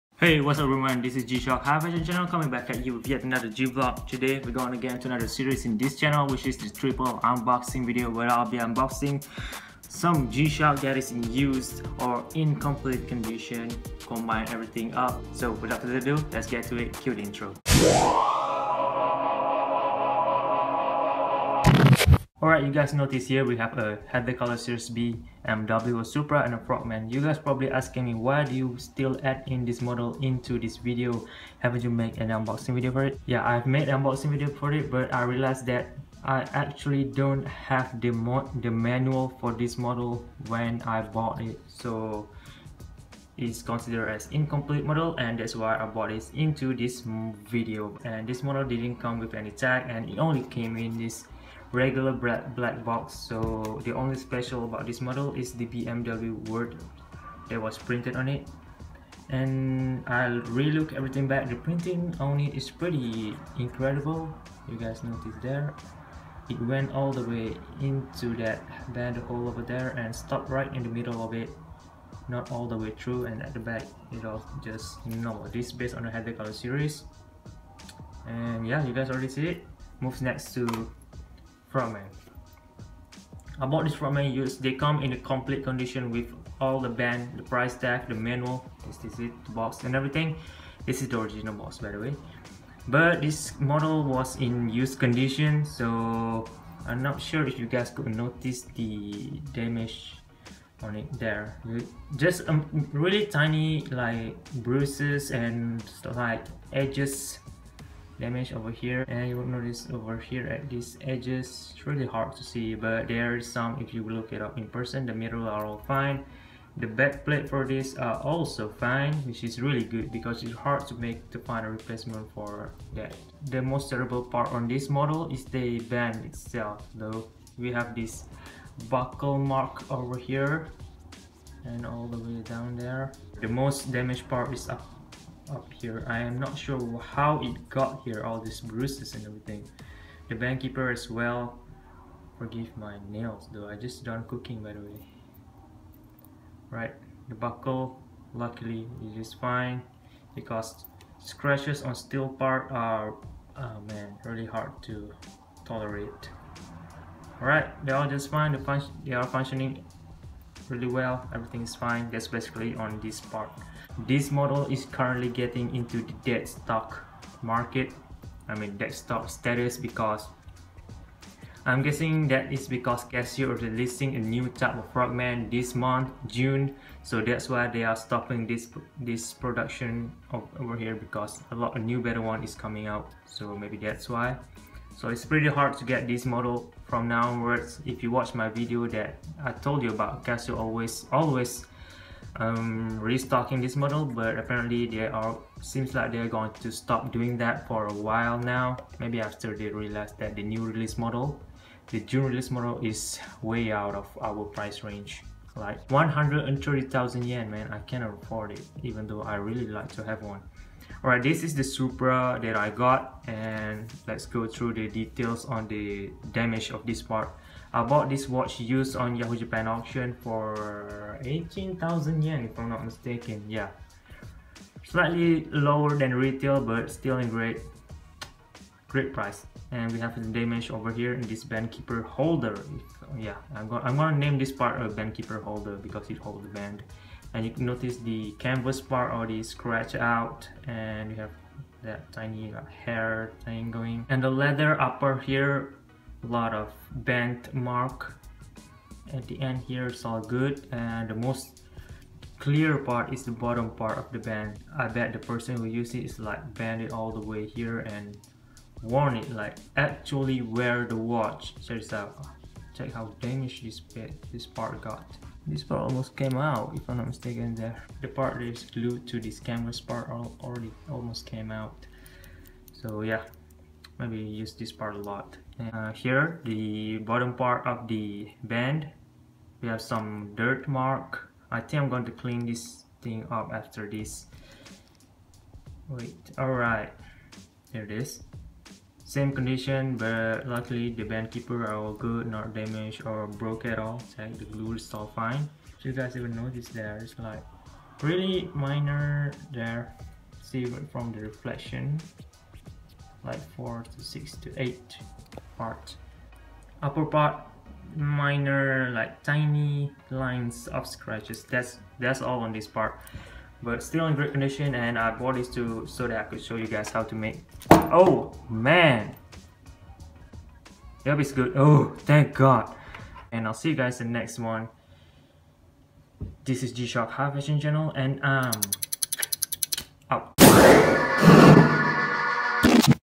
G-Shock triple unboxing | BMW X1 - FROGMAN - SUPRA
Its been a long time since the last time I did this type of video. Here it a simple and quick unboxing video of USED/WORN item and also items that are in in-complete condition. In this video I'll be covering up the recent pickup that I have which is the used basic black GWF-1000 frogman, the incomplete GA-110 heather color series BMW X1 limited and also the totally worn out GA-200SPR supra footwear model. Enjoy.

(Be the first to contribute)

All selling sites are now on hold because I'm not available at the moment for any of that matters. Sorry for any inconvenience.